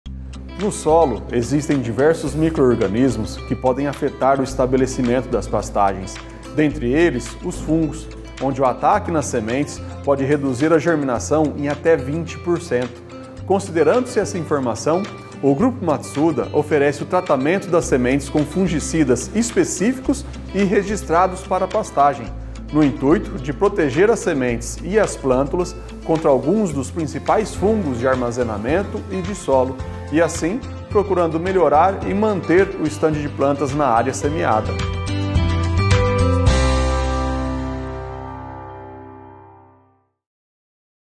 Sementes tratadas com fungicidas
Saiba mais sobre as sementes tratadas com fungicidas com o engenheiro agrônomo Pedro Henrique Lorençoni.